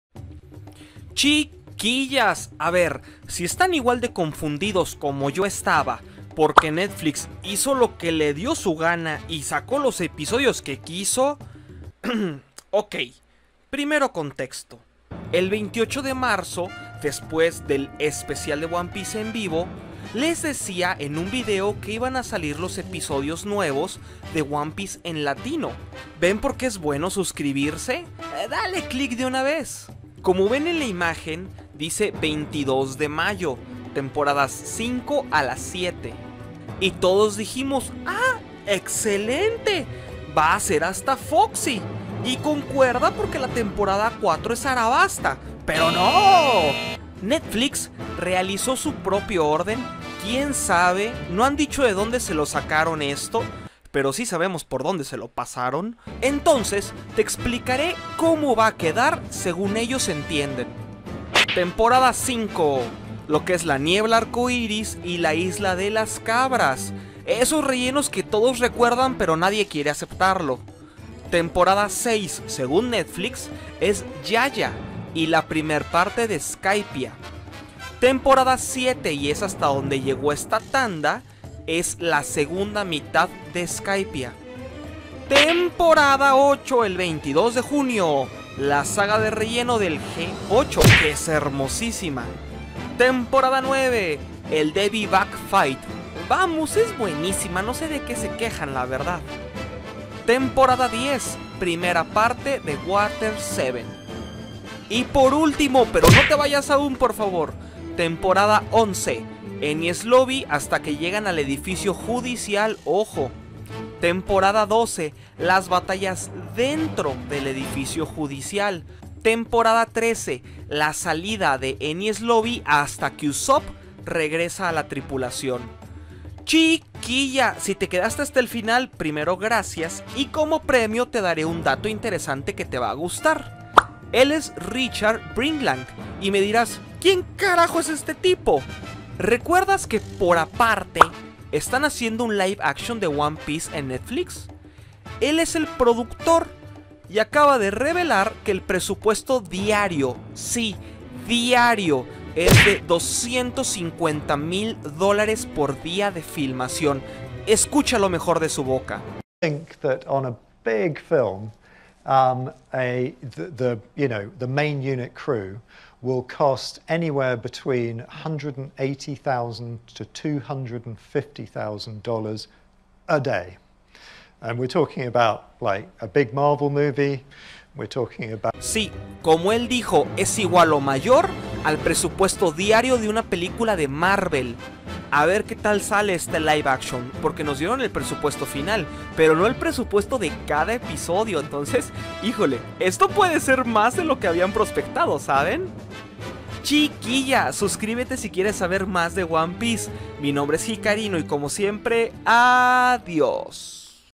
¿DEBERIAS VER LOS CAPITULOS NUEVOS? | ONE PIECE LATINO
Netflix realizo su propio orden, quien sabe, no han dicho de donde se sacaron esto... pero si sabemos por donde se lo pasaron...
entonces te explicare a como va a quedar según ellos entienden.
One Piece en Netflix latino.